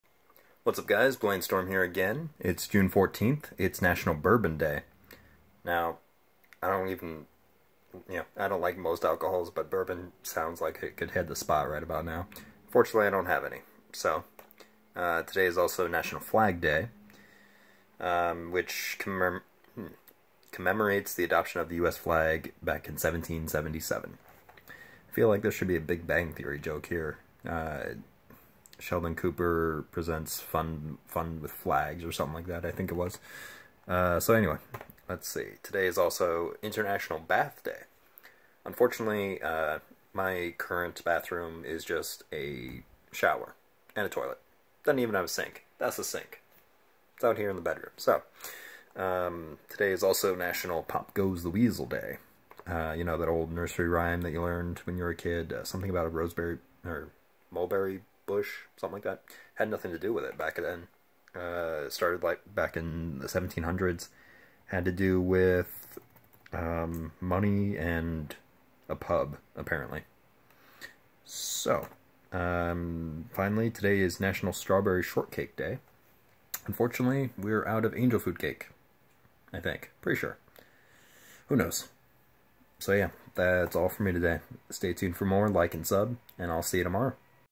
Storm 365 - June 14: National Flag Day
Not bats, BATHS. Taking a quick shower is just as refreshing and uses one-third the water. Celebrating National Bourbon Day, Flag Day, International Bath Day, Pop Goes the Weasel Day, and Strawberry Shortcake Day!